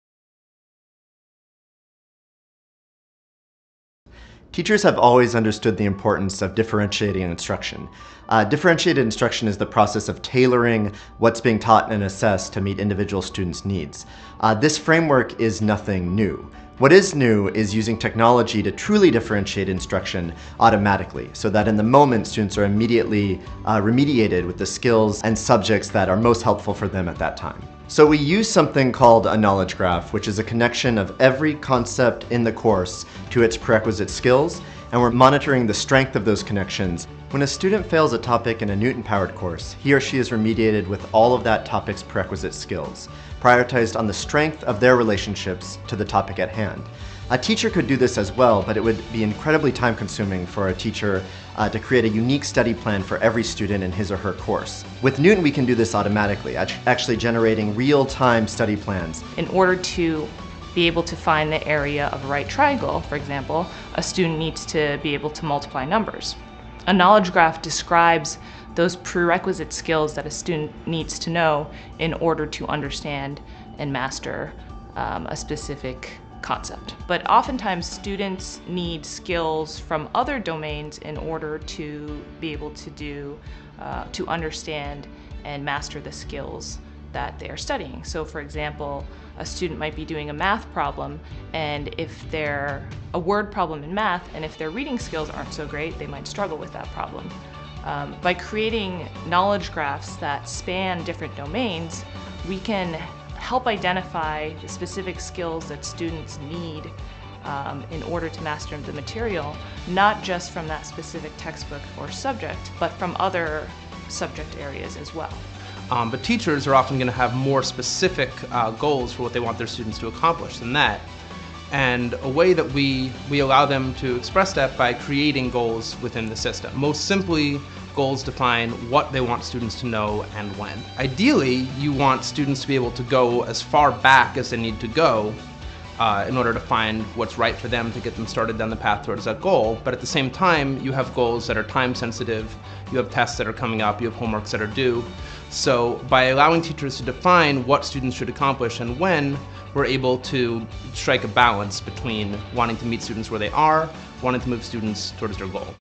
Adaptive Learning: Knewton
In this clip produced by Knewton, the company's content developers explain how they map learning concepts to prerequisite skills in order to create personalized learning paths. (From Campus Technology May 2014; Video courtesy of Knewton)